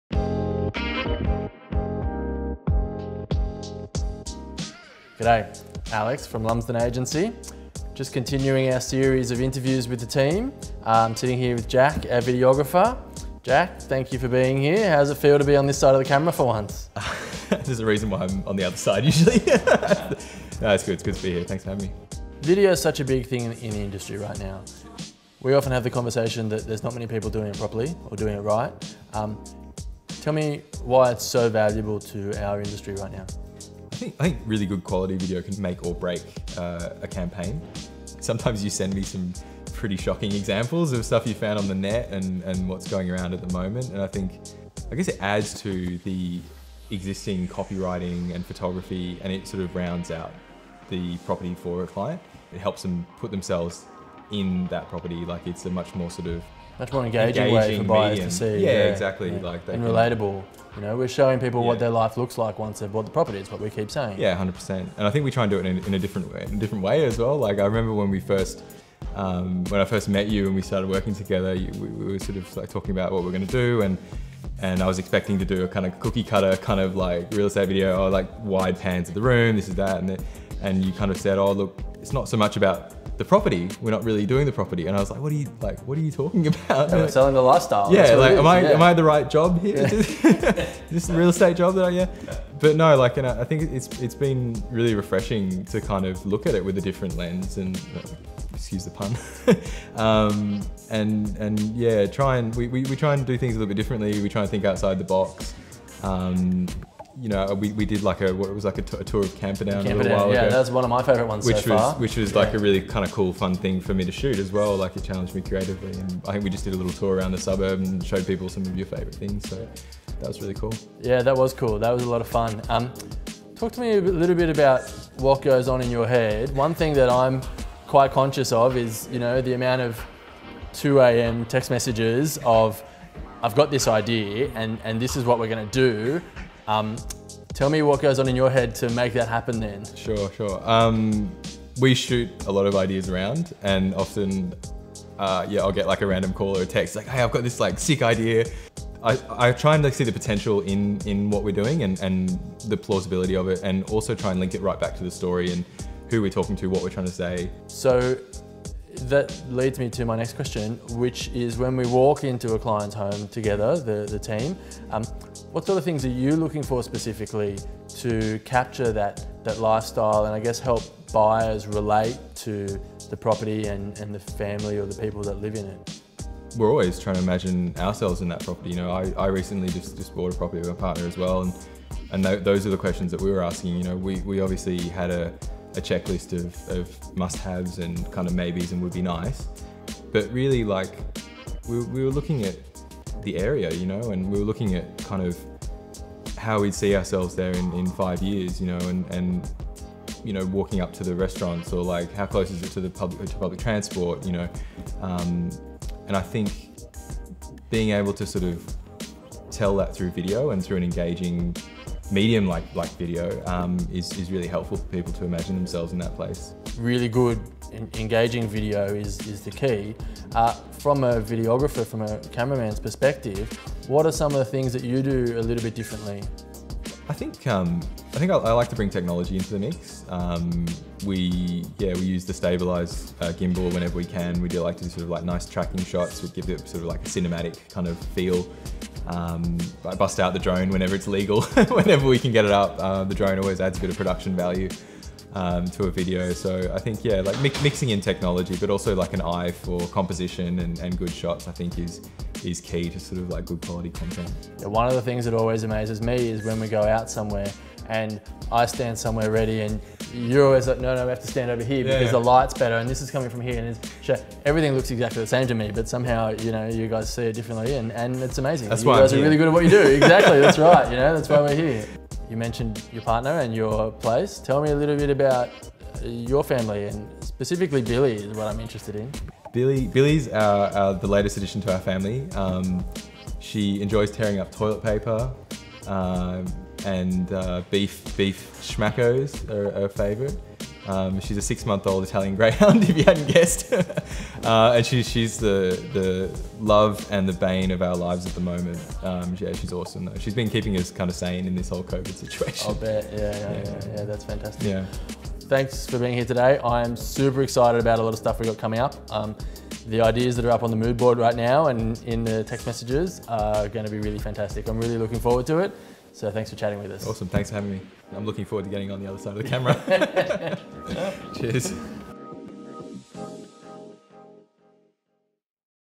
Introducing Jack Hibbert
Jack is our videographer and a Sydney Creative in every sense of the phrase. You can see it in his clothes, his home, and most certainly his work, and now he shares his thoughts on property marketing with us!